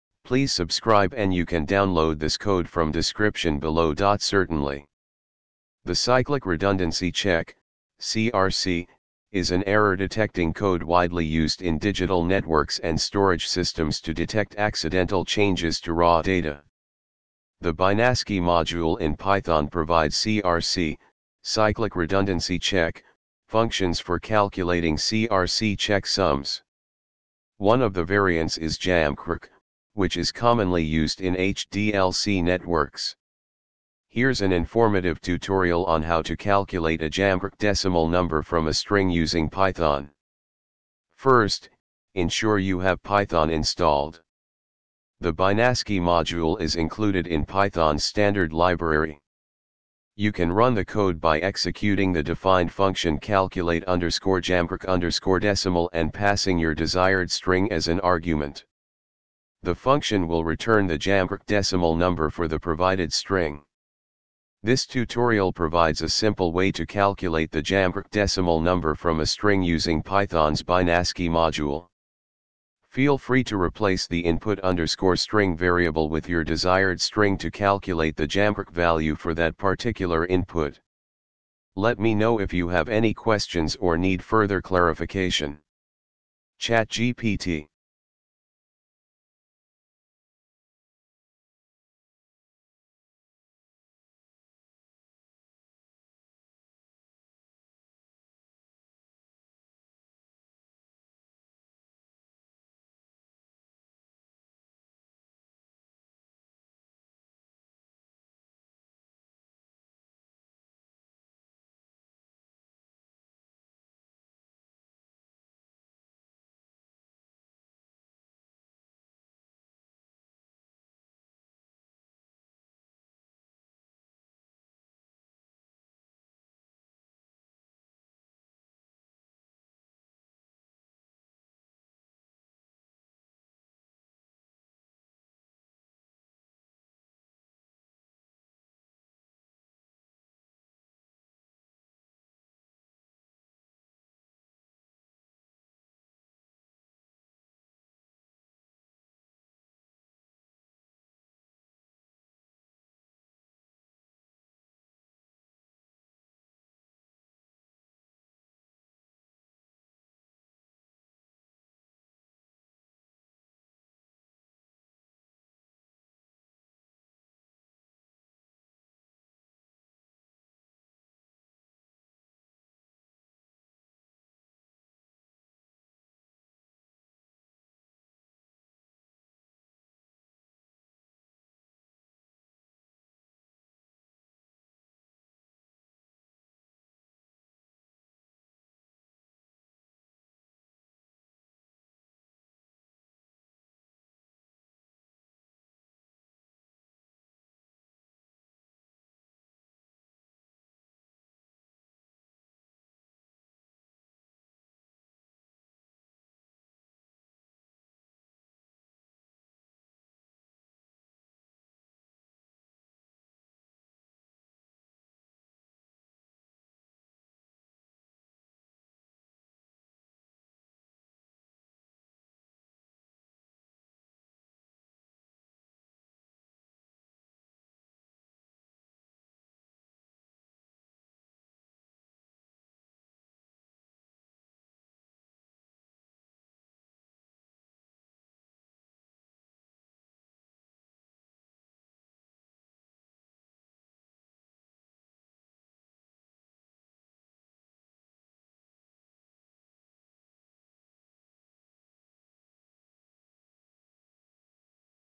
CRC Python How to calculate JAMCRC decimal number from string
Certainly! The Cyclic Redundancy Check (CRC) is an error-detecting code widely used in digital networks and storage systems to detect accidental changes to raw data. The binascii module in Python provides CRC (Cyclic Redundancy Check) functions for calculating CRC checksums. One of the variants is JAMCRC, which is commonly used in HDLC networks.
Here's an informative tutorial on how to calculate a JAMCRC decimal number from a string using Python:
First, ensure you have Python installed. The binascii module is included in Python's standard library.
You can run the code by executing the defined function calculate_jamcrc_decimal and passing your desired string as an argument. The function will return the JAMCRC decimal number for the provided string.
This tutorial provides a simple way to calculate the JAMCRC decimal number from a string using Python's binascii module.
Feel free to replace the input_string variable with your desired string to calculate the JAMCRC value for that particular input.
Let me know if you have any questions or need further clarification!
ChatGPT